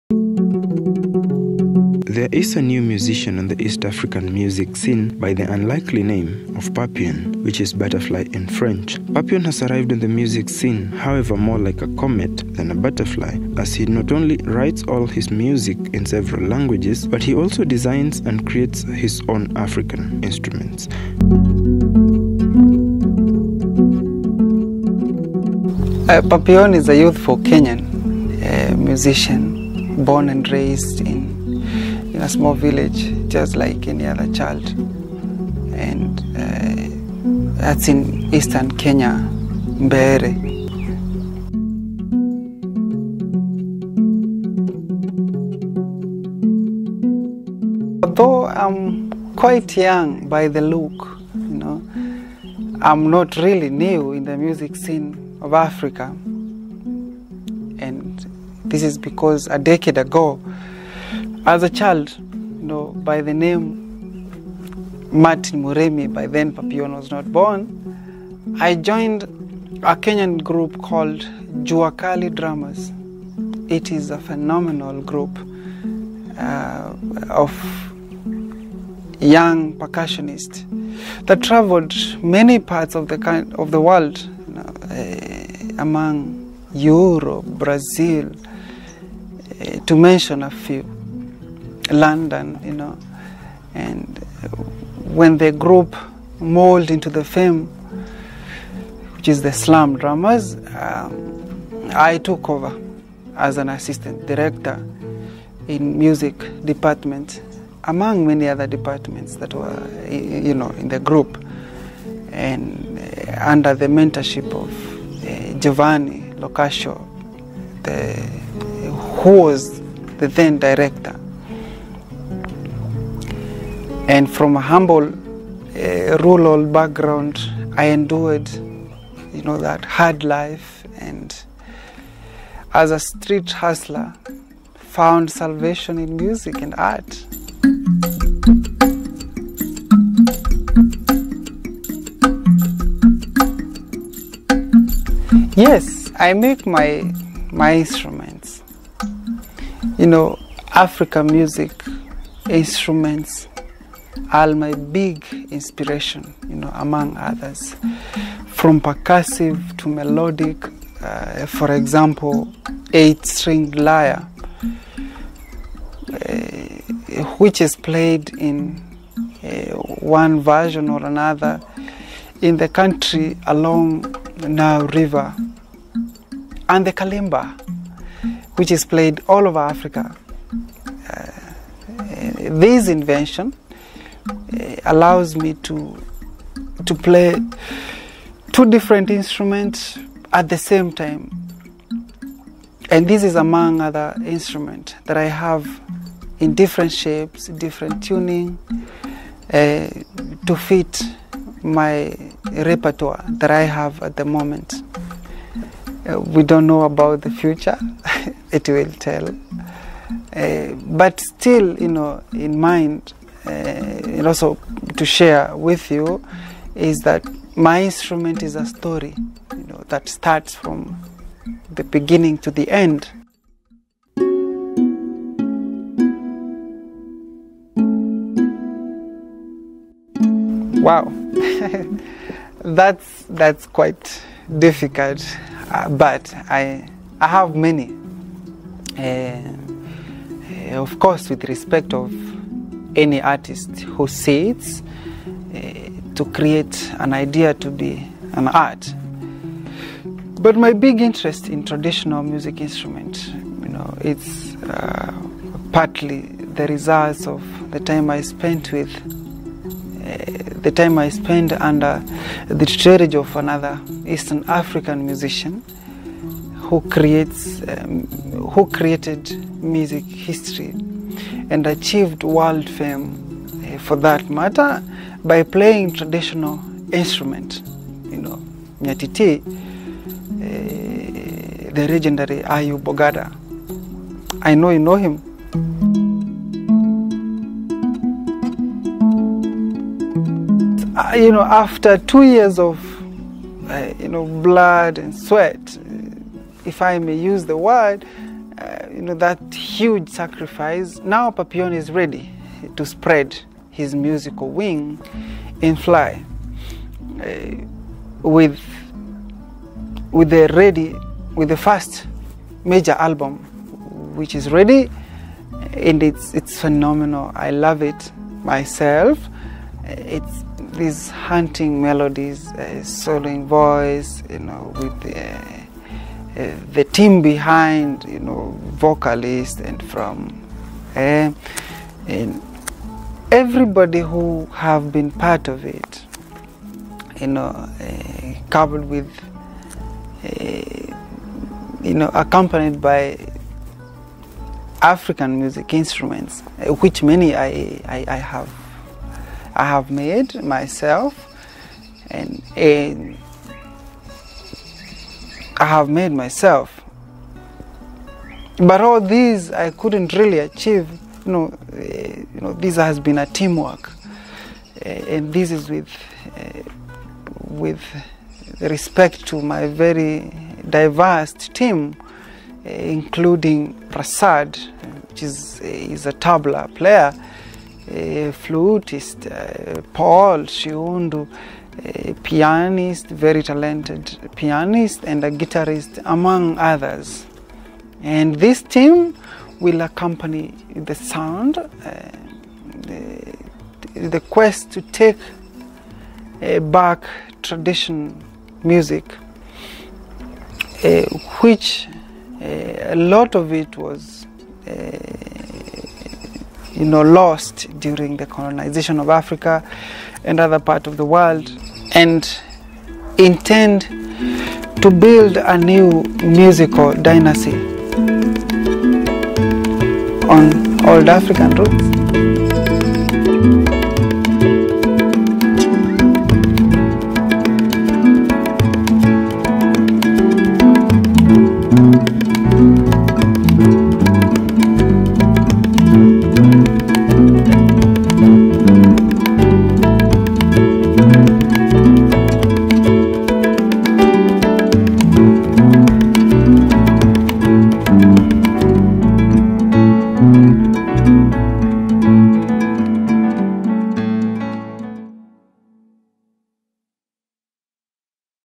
Papillon Visual Profile
This is the story of my musical journey.
From a humble rural background, I endured a hard life, and found salvation in music and art.